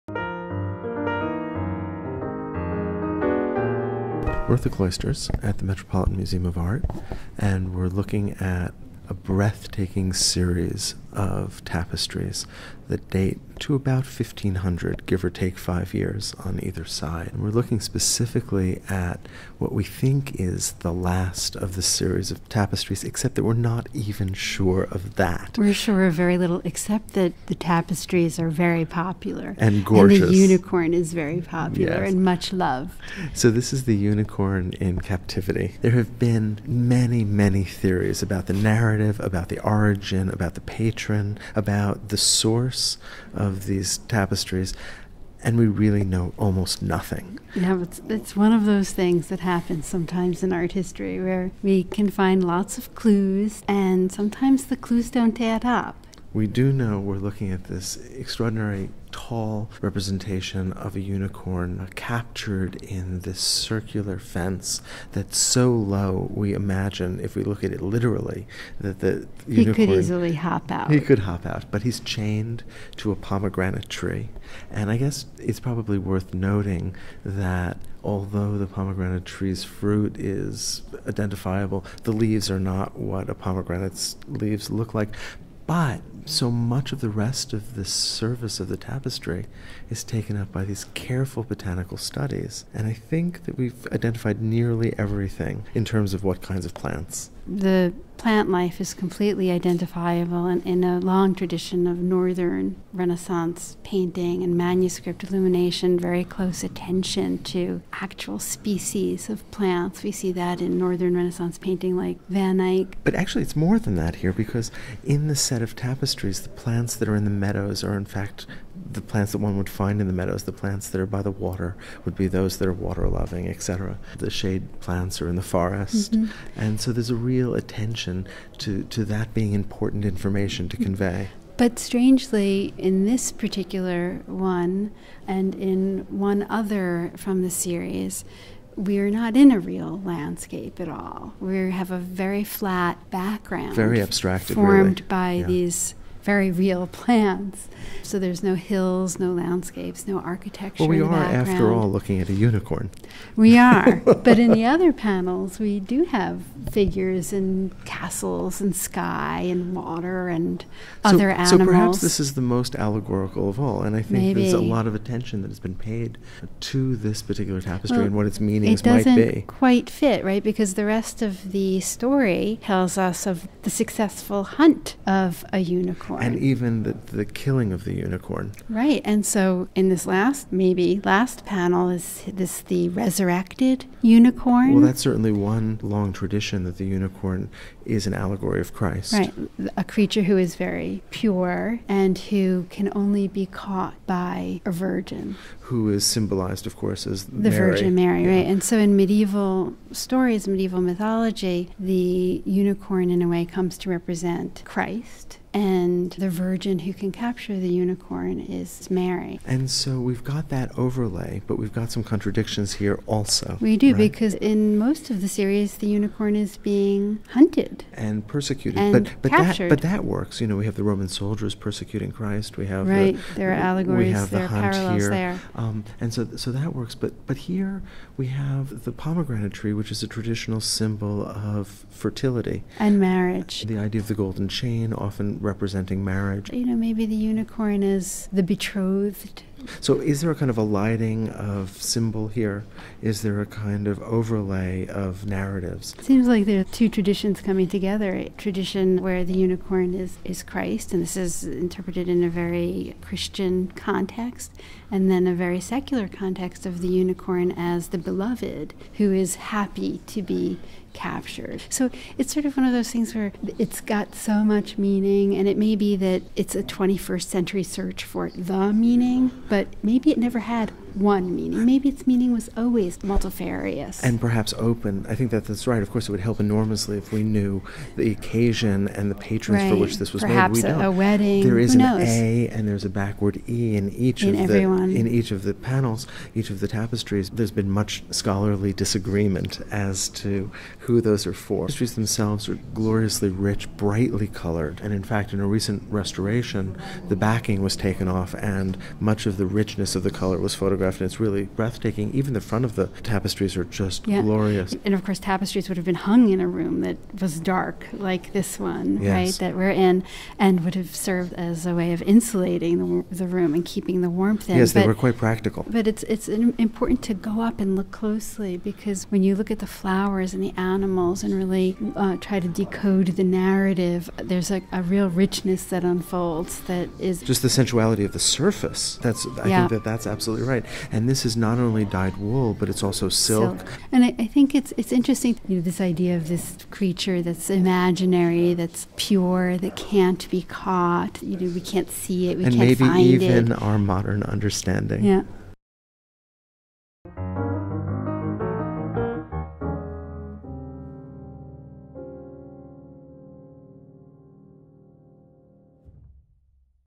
The Unicorn in Captivity
The Unicorn in Captivity (one of seven woven hangings popularly known as the Unicorn Tapestries or the Hunt of the Unicorn), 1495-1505, South Netherlandish, wool, silk, silver, and gilt (The Cloisters, The Metropolitan Museum of Art). Created by Beth Harris and Steven Zucker.